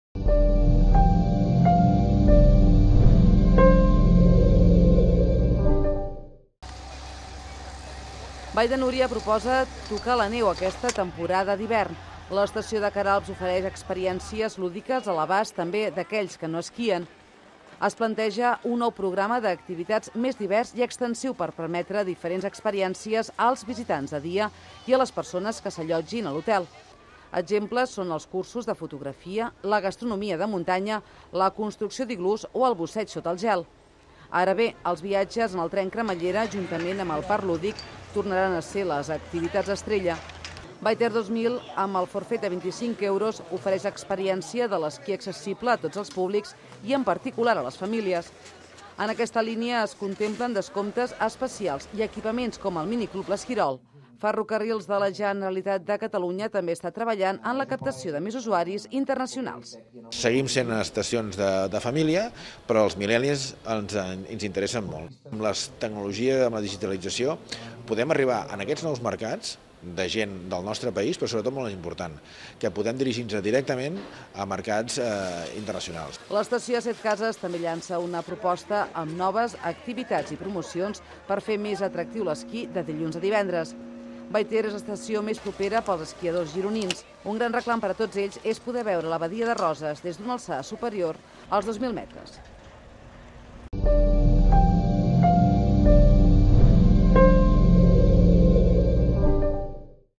FGC presenta les novetats de la temporada d'hivern
Preparatius d’una temporada d’hivern que es presenta apassionant a les estacions del Ripollès Ferrocarrils de la Generalitat de Catalunya ja ha presentat les novetats dirigides a tots els públics.